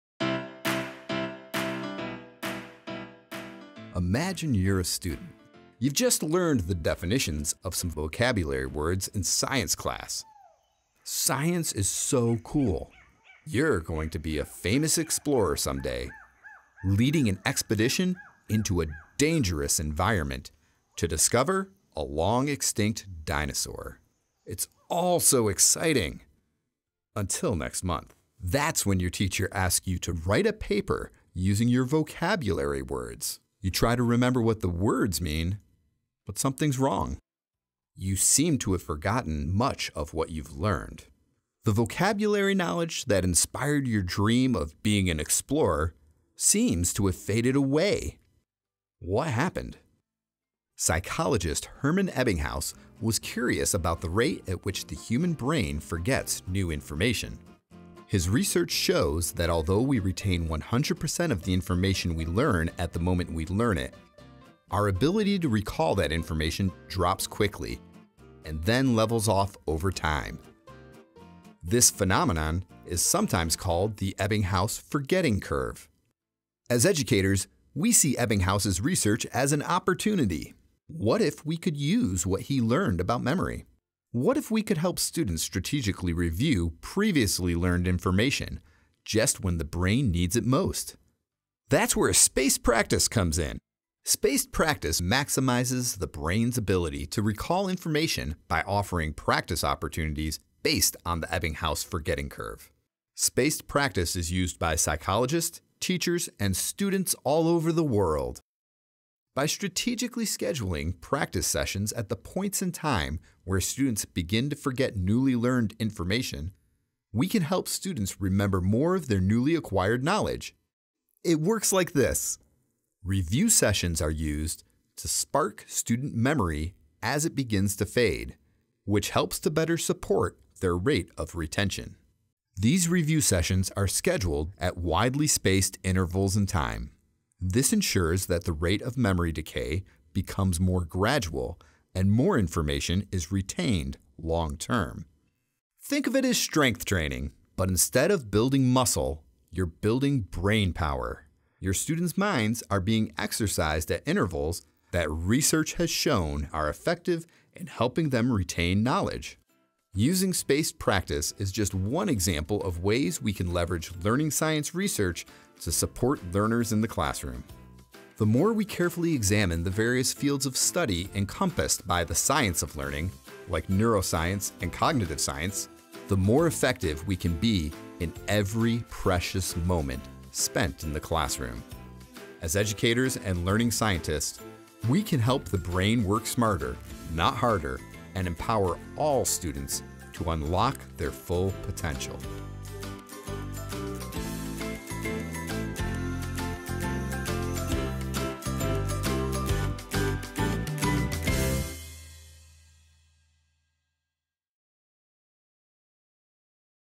Teaching Strategies: Spaced Practice
Our teaching strategies video series uses research to help educators understand how students learn, so they can incorporate instructional methods and practices in their classroom. In our first installment, we explored information processing and schemas through the Cognitive Load Theory. Now, we're taking a look at memory and studying with Spaced Practice.